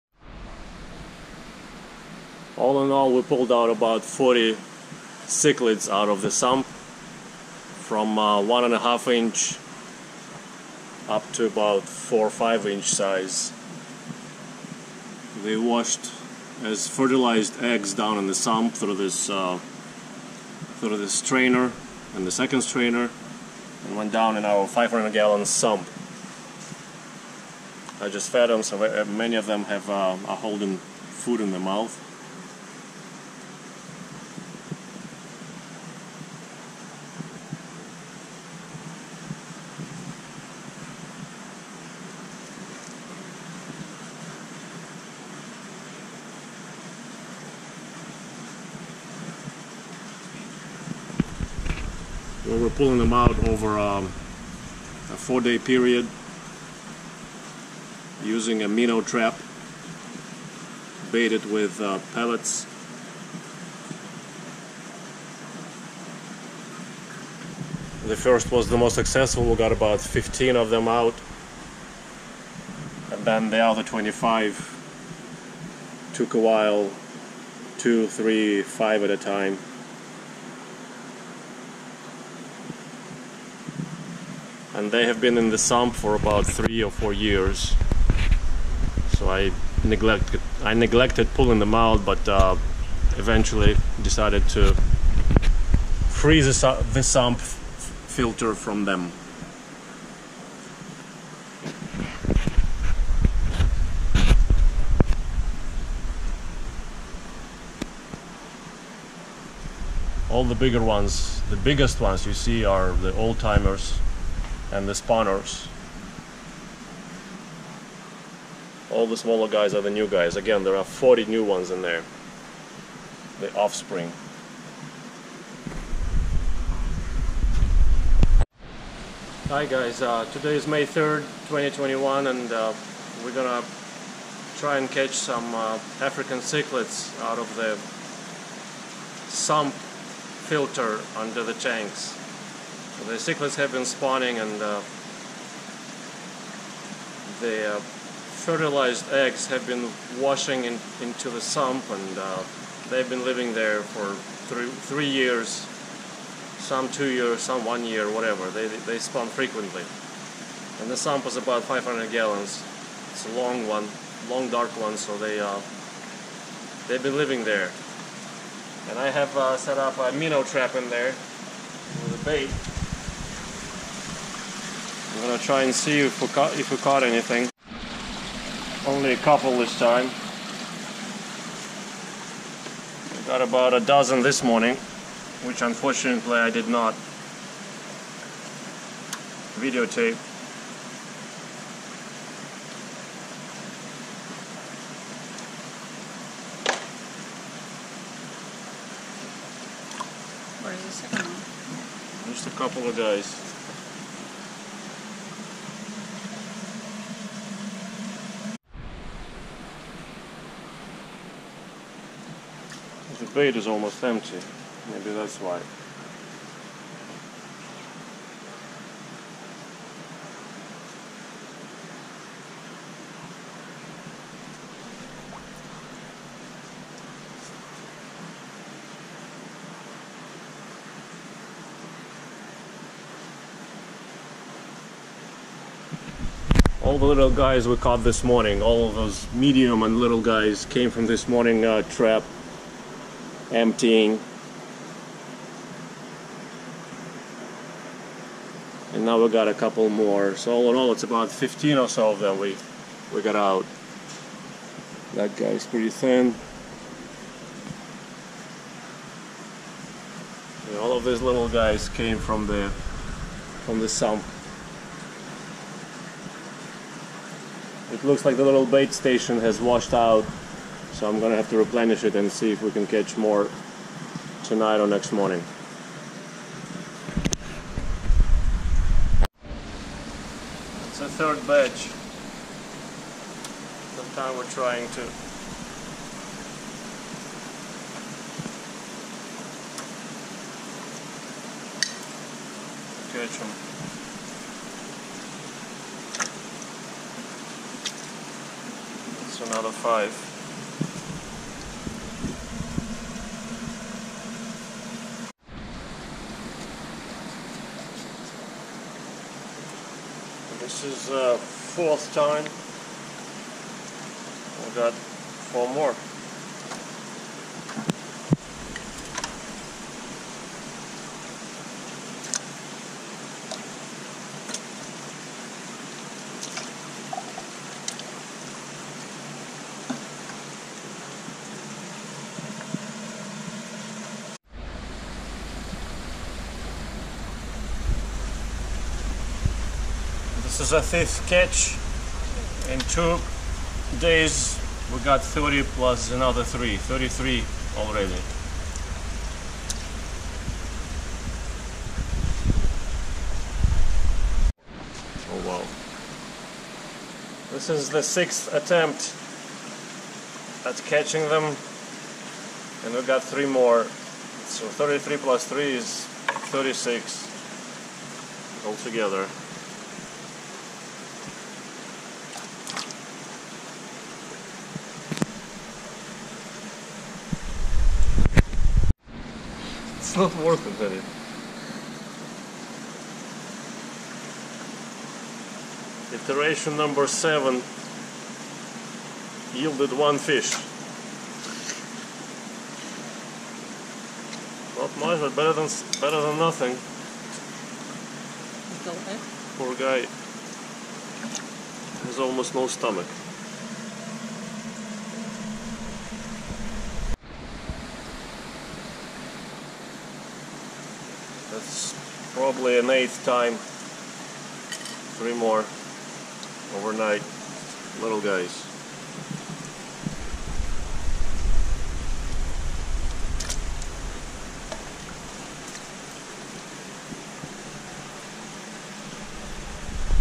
40 more jerks from sump filter for the jerk tank, may not sound glamorous but it's business as usual
Fish Story Pubic Aquarium & Fish Rescue
316 Morgan Rd
Naples, Florida 34114
USA